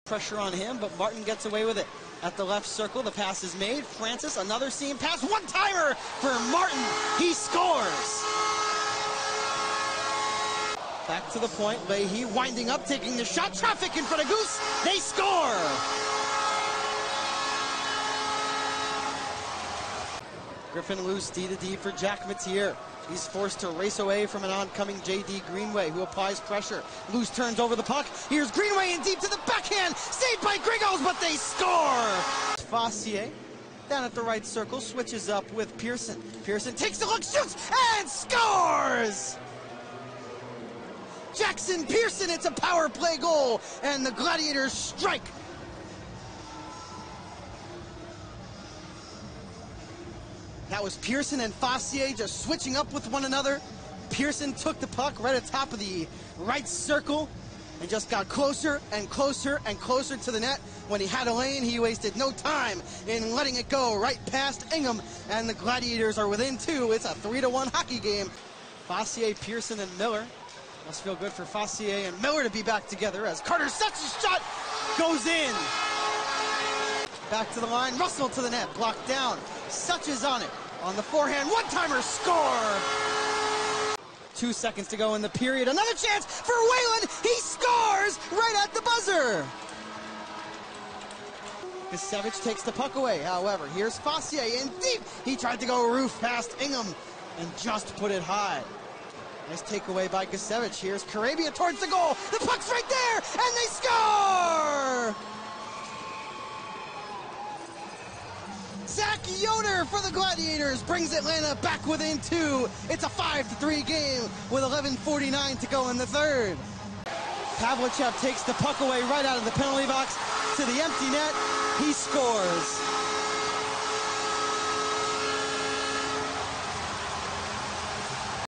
Atlanta Gladiators (3) @ Greenville Swamp Rabbits (6) (12-1-2023)
The Atlanta Gladiators’ (9-7-0-0) gallantly tried to battle back from an early three-goal deficit but ultimately came up short as the Greenville Swamp Rabbits (14-2-0-0) escaped with a 5-3 victory Friday evening at Bon Secours Wellness Arena in Greenville, South Carolina.